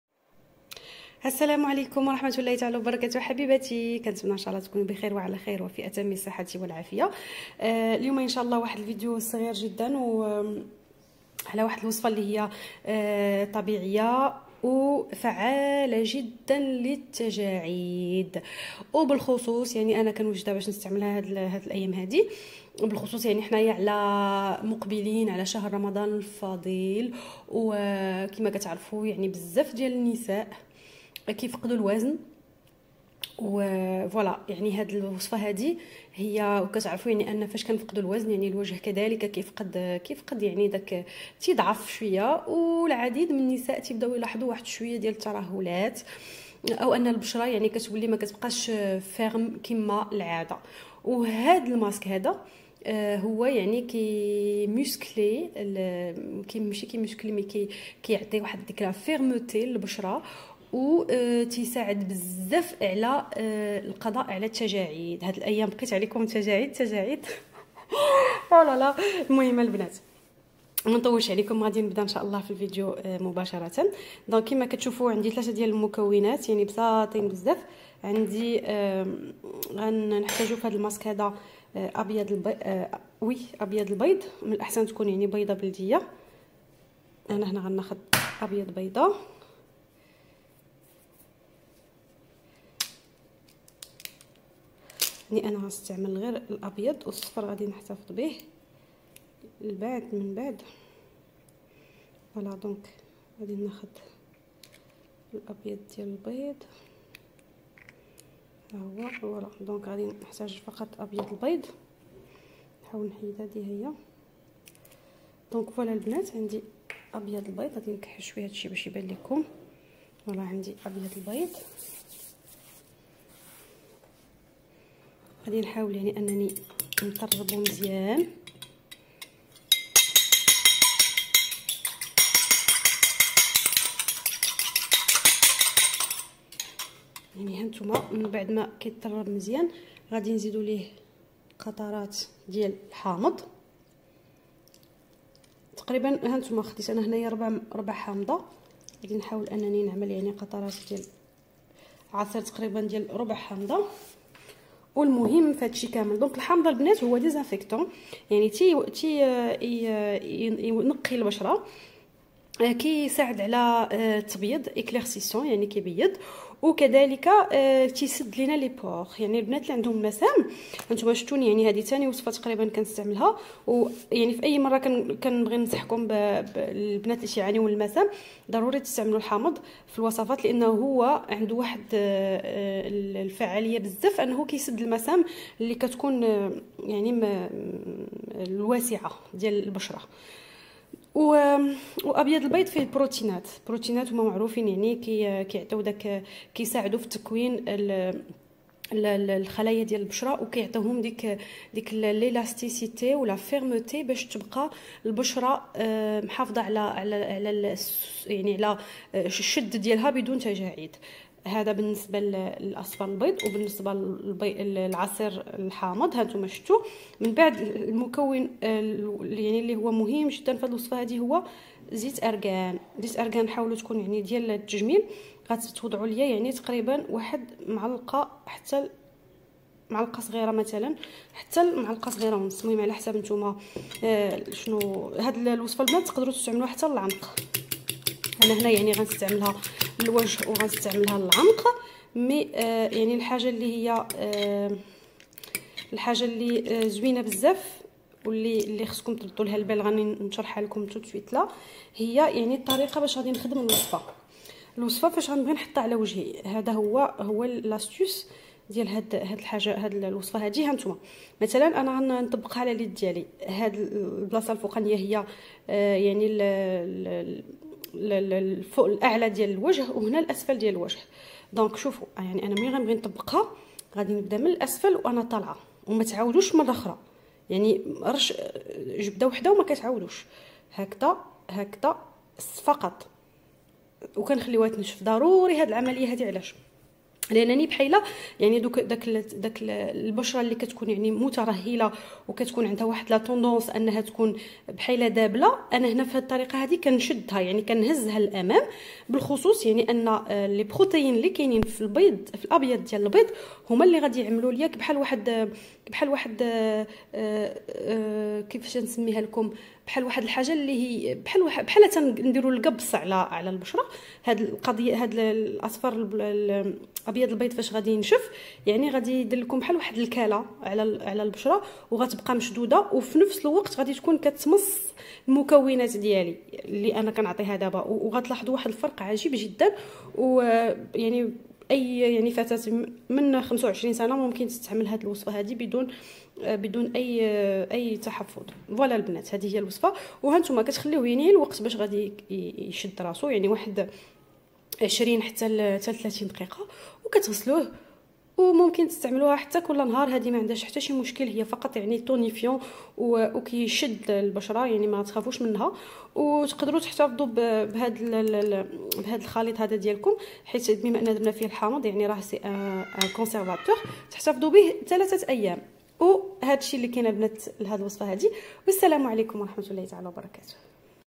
وصفة طبيعية لشد البشرة و محاربة التجاعيد
و صفة طبيعية لشد البشرة و محاربة التجاعيد
بابيض البيض : الغني بالبروتينات التي تساهم في تكوين الكولاجين.
عصير الحامض الذي يساعد على سد المسام غني بڤيتامين C  المهم جدا لشباب و نضارة البشرة + مطهر و منظف
زيت ارݣان الغني عن التعريف و أقوى زيت لمحاربة التجاعيد و الغني بمضادات الاكسدة و الفيتامينات.